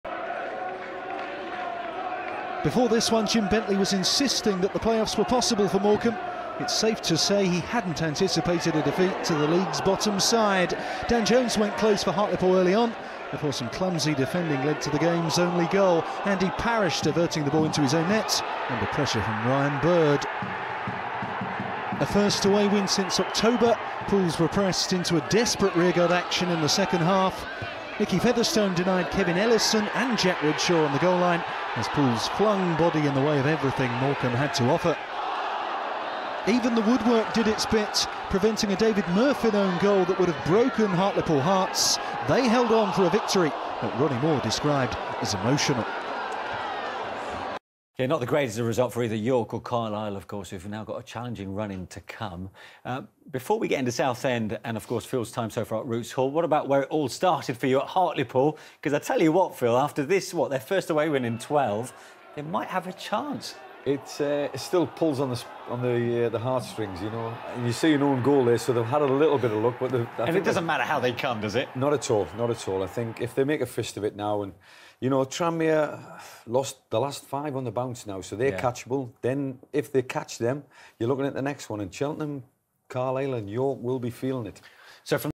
Morecambe 0-1 Hartlepool United - 14th March 2015
(0-1) Andy Parrish og 35'

HUFC: Scott Flinders, Michael Duckworth, Scott Harrison, David Mirfin, Dan Jones, Neil Austin, Brad Walker (Lewis Hawkins 72), Nicky Featherstone, Aaron Tshibola, Jonathan Franks, Ryan Bird // Manager - Ronnie Moore

MFC: Andreas Arestidou, Shaun Beeley (Aaron McGowan 86), Mark Hughes, Andy Parrish, Laurie Wilson, Jack Redshaw, Andrew Fleming, Andrew Wright (Stewart Drummond 65), Kevin Ellison, Padraig Amond, Paul Mullin (Jack Sampson 66) // Manager - Jim Bentley